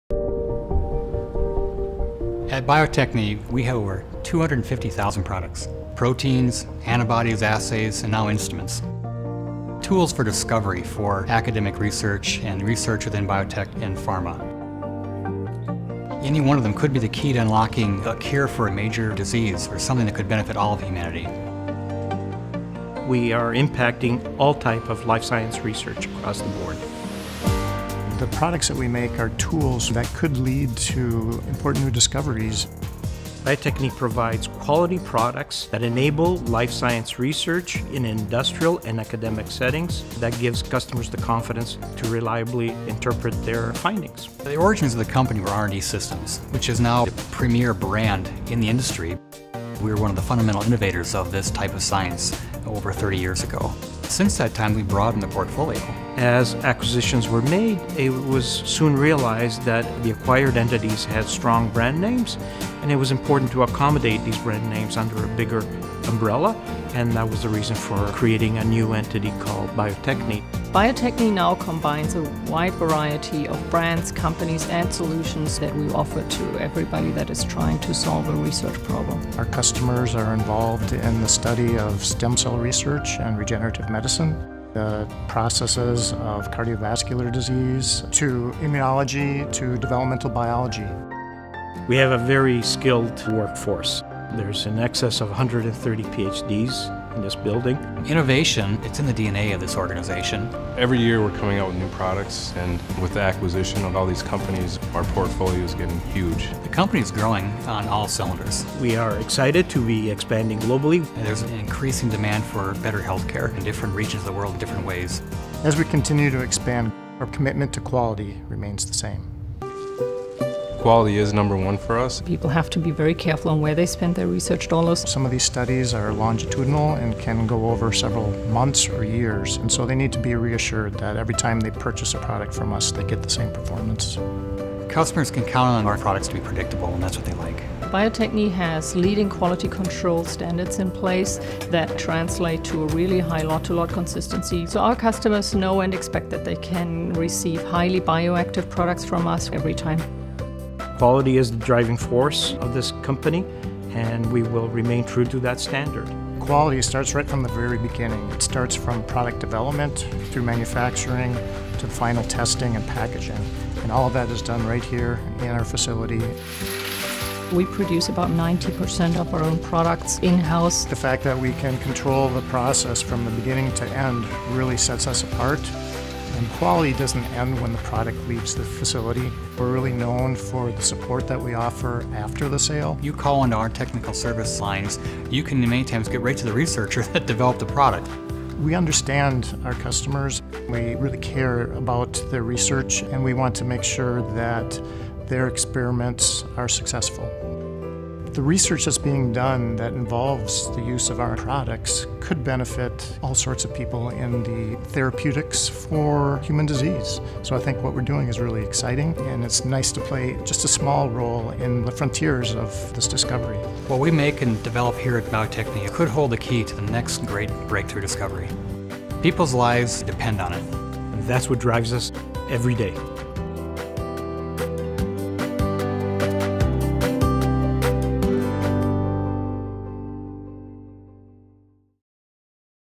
A Reputation of Quality: The Bio-Techne Brand Story
Bio-Techne is built on a legacy of quality. R&D Systems, Novus Biologicals, Tocris, ProteinSimple, Advanced Cell Diagnostics and BiosPacific have been brought together as Bio-Techne to be a stronger scientific partner to help customers attain their research goals. Our mission is to provide the Life Sciences community with innovative, high-quality scientific tools to better understand biological processes and drive discovery. Watch our story here.